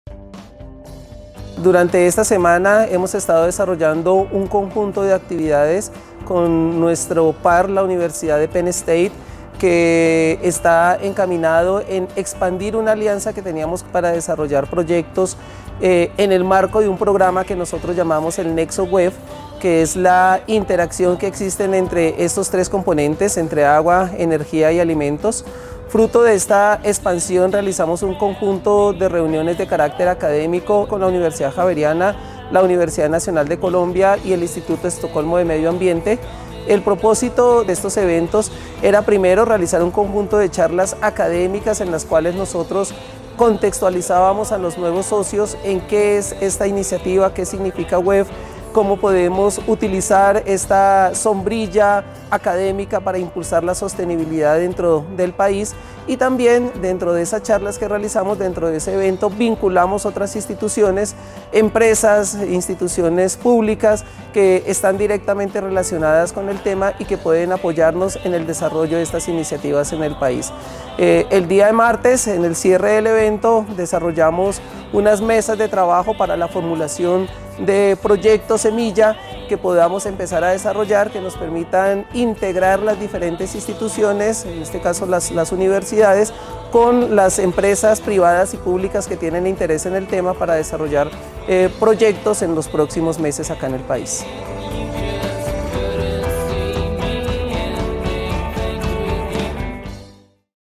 Alianzas para construir un mejor país con un enfoque WEF
Durante una semana se realizaron charlas académicas en el marco del programa WEF Nexus, la interacción entre agua, energía y alimentos. El objetivo fue dar a conocer el proyecto a nuevos aliados. El centro de Biosistemas hizo parte del recorrido. Allí Rodrigo Gil, el director del centro, habló de cómo esas alianzas le apuestan a la sostenibilidad.

Oficina de Comunicación
Uso exclusivo de la Universidad Jorge Tadeo Lozano para publicaciones impresas o digitales

CRÉDITOS
🎥  💻 ©️ Katerina Rincón Jiménez / Oficina Comunicaciones Utadeo